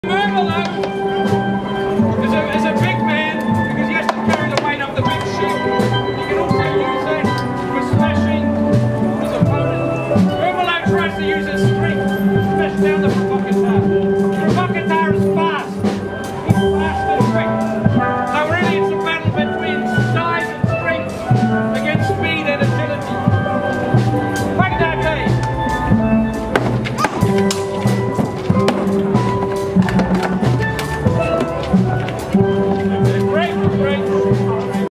Murmillo versus Provocator. Roman Gladiator demo at Cambridge Autumn Festival - Sun 17 April 2016
The Romans give a demonstration to the public of various gladiator styles and fighting techniques. Murmillo Marcus Carnifex (the Butcher) (Mitch Green) versus Petronius the Pict (Pete Rogers).